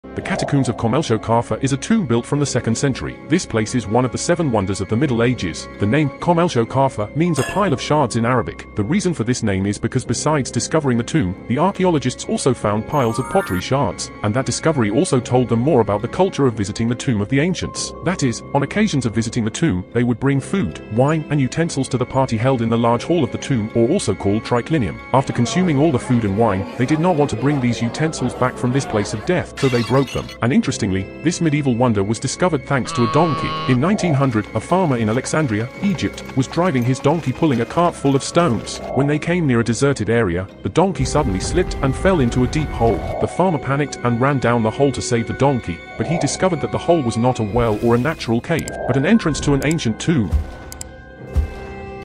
ArchiDiscover | Mysterious tomb in Alexandria, Egypt.
Do you know what The Catacombs of Kom el Shoqafa is? This is an ancient catacomb located in Alexandria, Egypt, and is one of the seven wonders of the Middle Ages. This catacomb has many interesting mysteries and stories, such as its name, its architectural style, and how it was discovered. Join me in exploring The Catacombs of Kom el Shoqafa in this short video. 😊